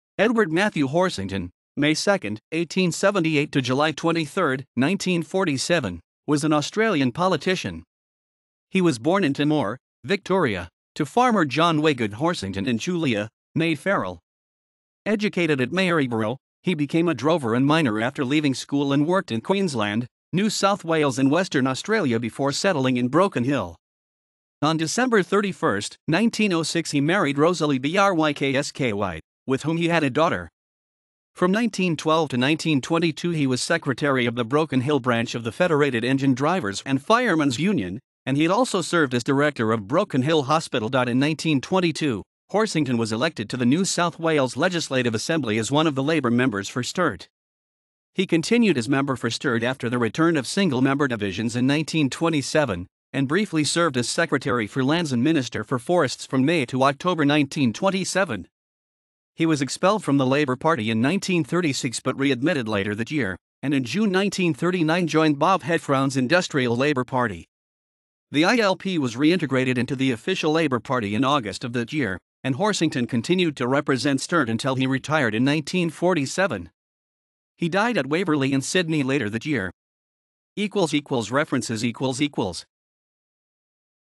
Ted Horsington
Edward Matthew Horsington (2 May 1878 – 23 July 1947) was an Australian politician.
He was born in Timor, Victoria, to farmer John Waygood Horsington and Julia, née Farrell. Educated at Maryborough, he became a drover and miner after leaving school and worked in Queensland, New South Wales and Western Australia before settling in Broken Hill. On 31 December 1906 he married Rosalie Bryksky, with whom he had a daughter. From 1912 to 1922 he was secretary of the Broken Hill branch of the Federated Engine Drivers and Firemen\s